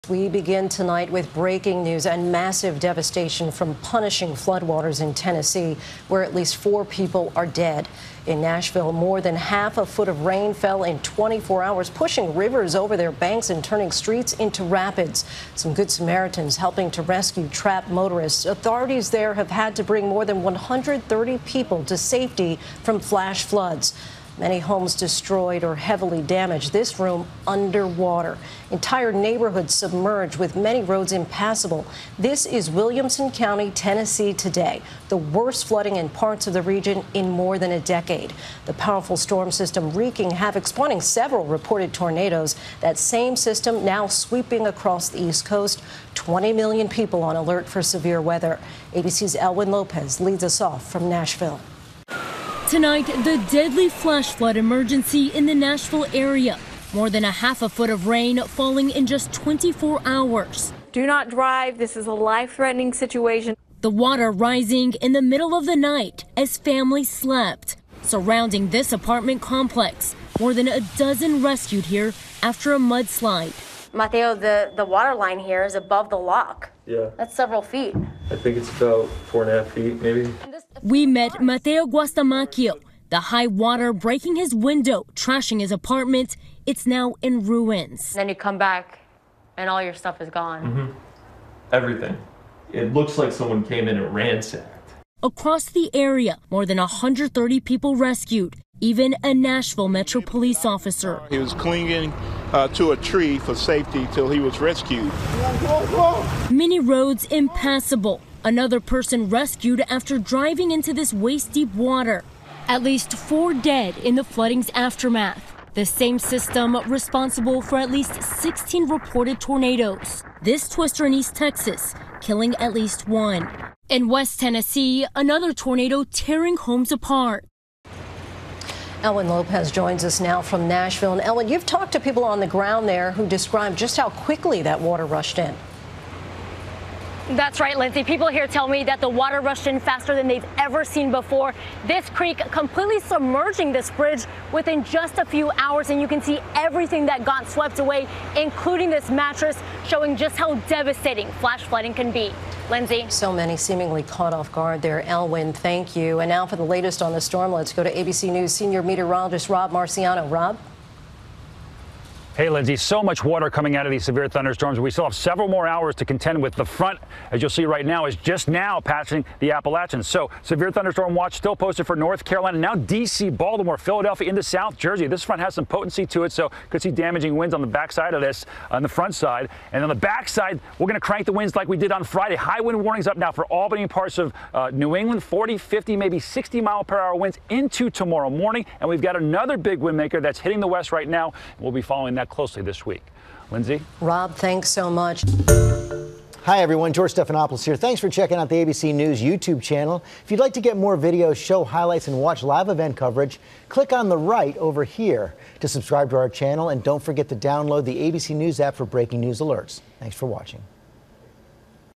Flood waters cause massive devastation in Tennessee
In Nashville, more than half a foot of rain has fallen in just 24 hours.